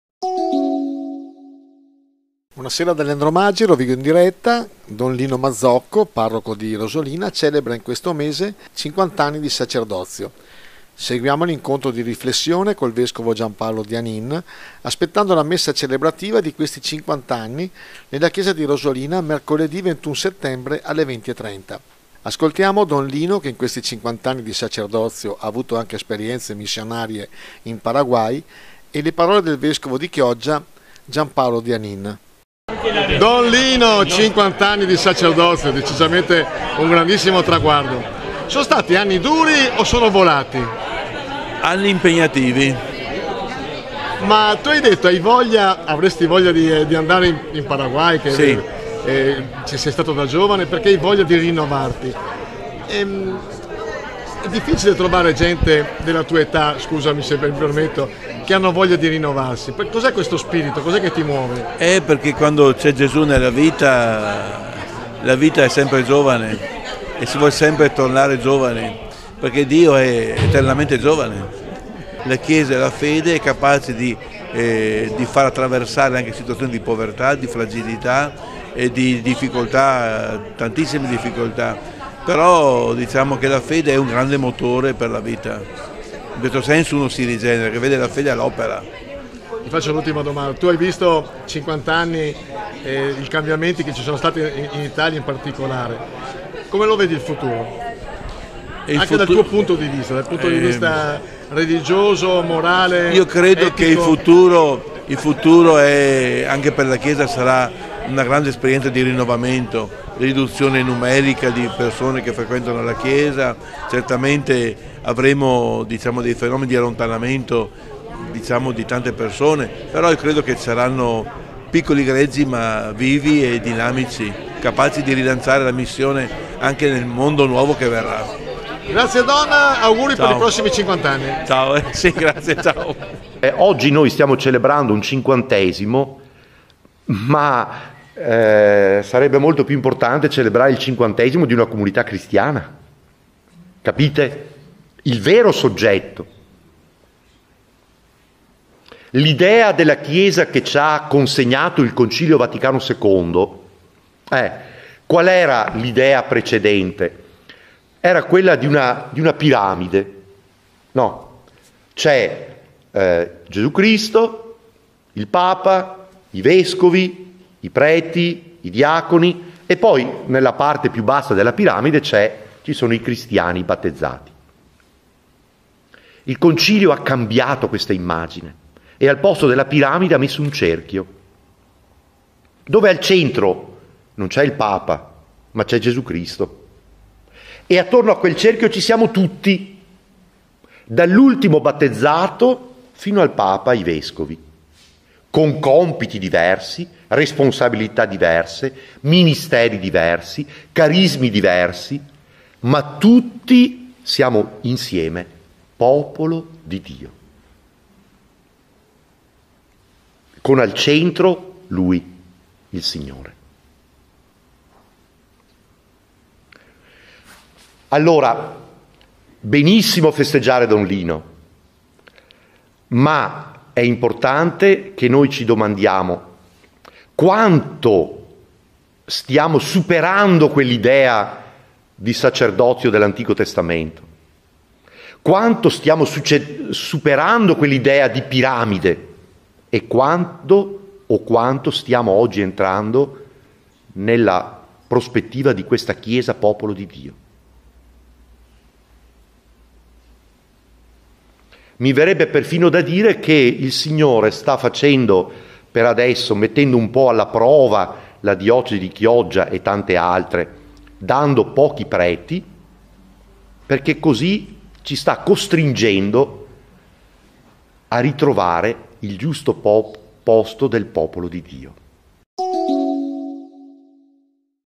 Rosolina - Grande festa per i 50 anni di sacerdozio di Don Lino
Don Lino, mezzo secolo d’amore. Grande festa per i 50 anni di sacerdozio: "Qui ho trovato ciò che cercavo, ma sogno il Paraguay"